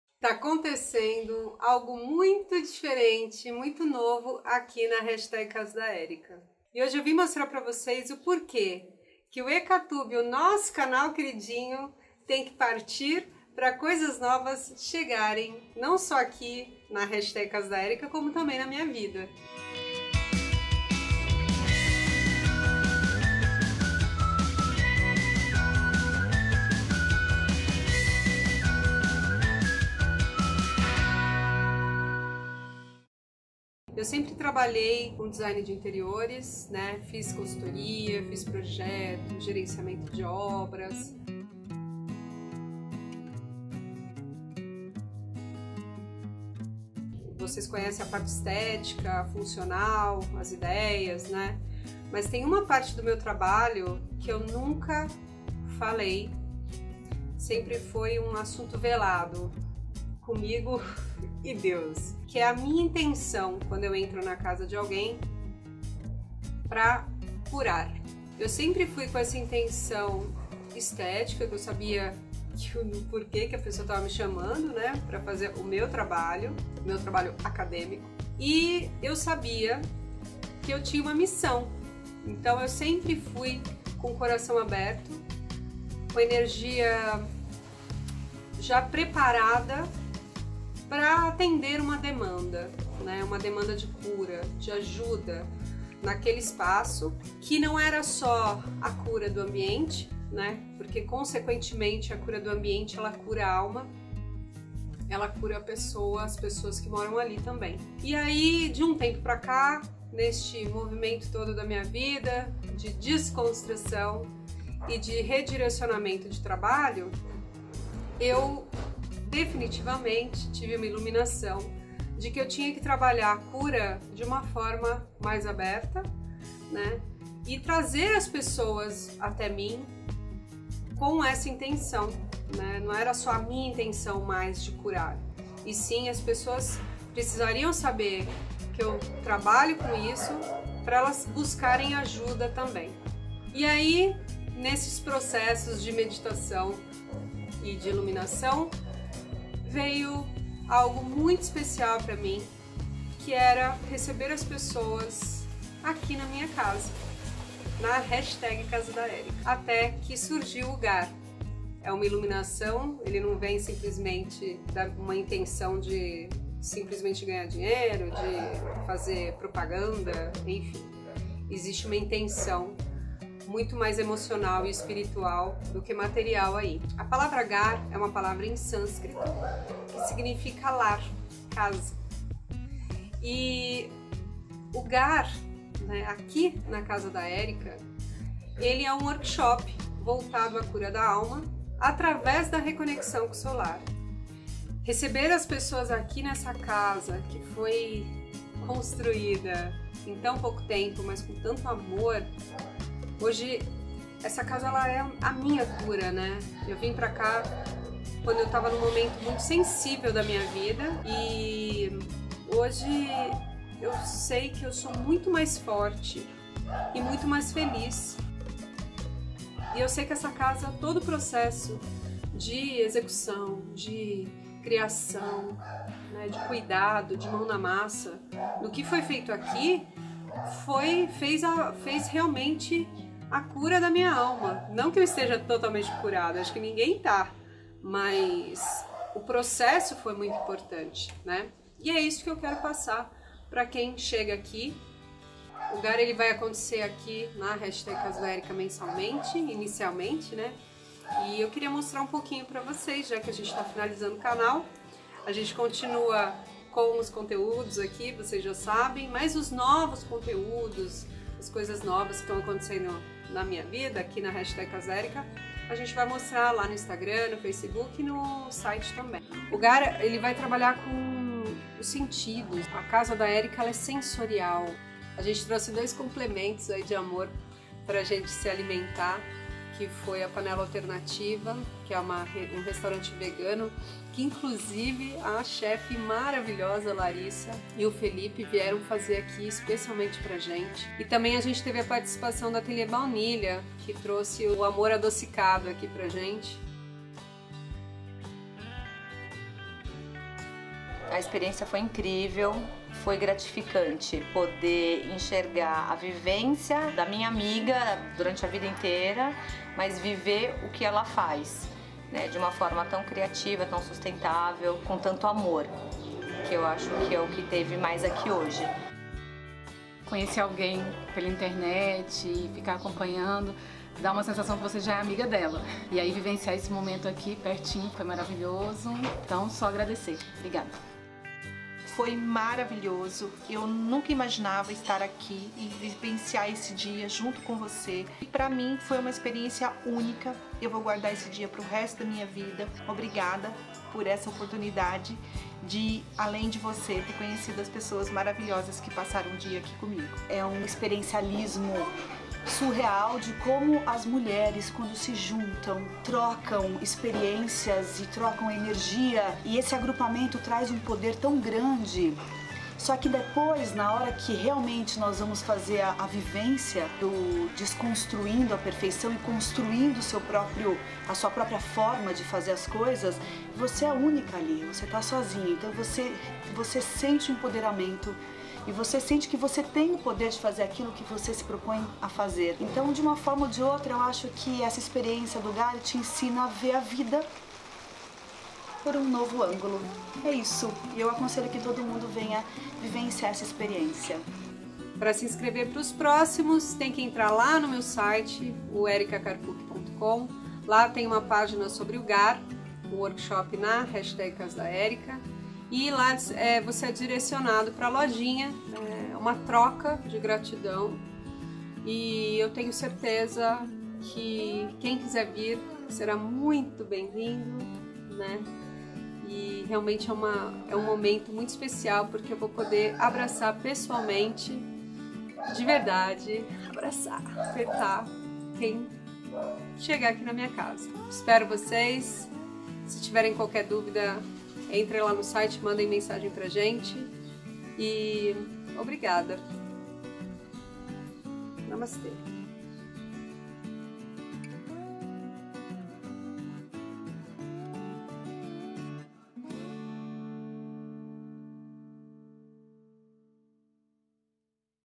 GHAR, A CURA através da RECONEXÃO com seu LAR
RECONEXÃO GHAR घर - A casa como ferramenta para o autoconhecimento
Ghar se escreve assim: घर, e significa Lar em sânscrito.

O Reconexão Ghar é um convite para retornar ao nosso lar interno e redescobrir nossa essência por meio do espaço físico que habitamos.

Lançado em 2019, o curso ganhou novo significado durante a pandemia de Covid-19, quando a casa passou a ser vista como uma extensão essencial do ser.

Adaptando-se às novas necessidades, o Reconexão Ghar se expandiu para palestras e consultorias online, acolhendo milhares de pessoas em sua jornada de transformação e conexão mais consciente com o lar.

Atualmente o Reconexão Ghar é um dos conceitos que direciona meu trabalho, em meus projetos e criações. Aqui, você poderá se aprofundar nos assuntos, e se surpreender com o poder de transformação que o Reconexão Ghar tem ♥
घर